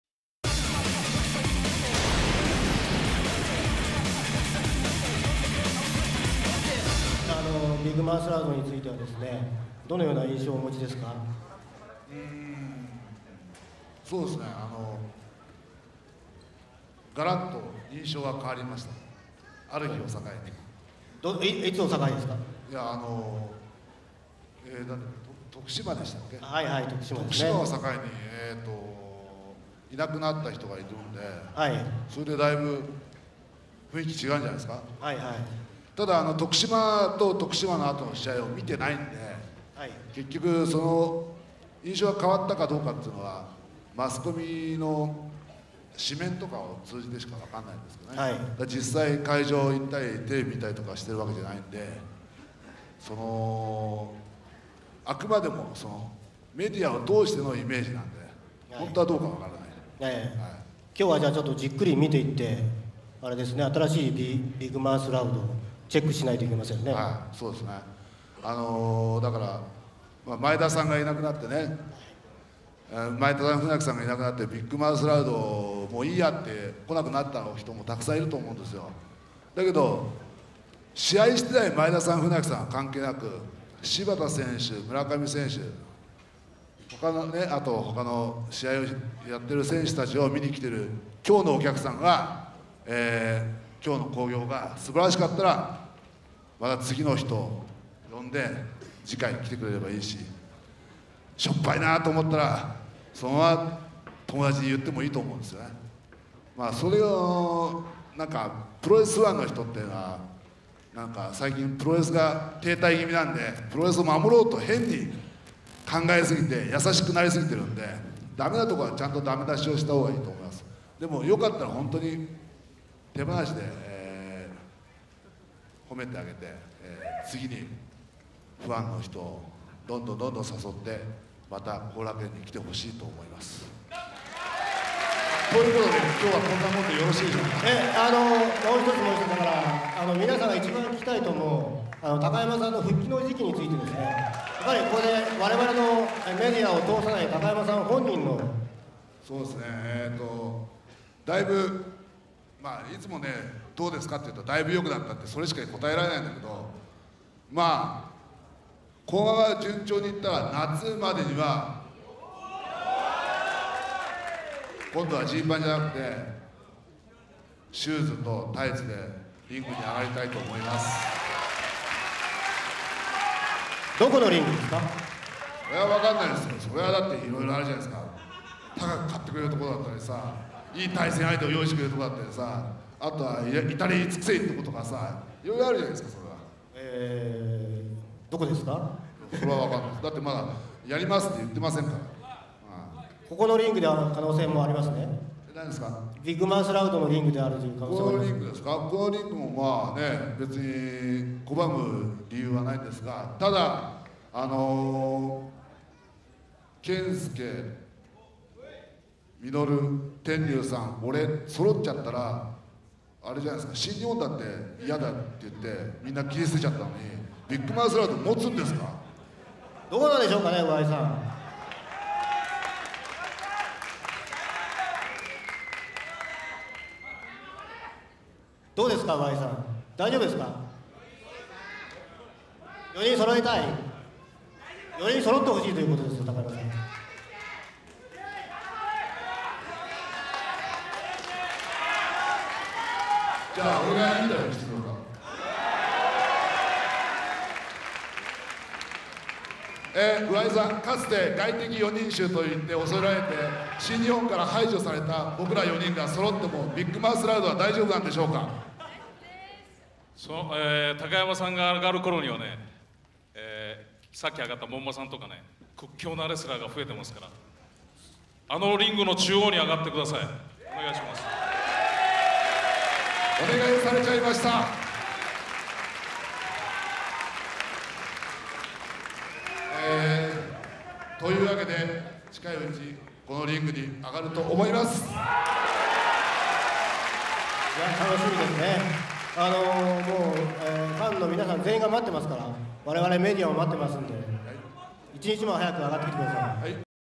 BML - Yoshihiro Takayama vs Soichi Shibata (Talk Battle)
Big Mouth LOUD talk battle - "Illusion 4" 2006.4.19

高山善廣VS柴田惣一 (トークバトル)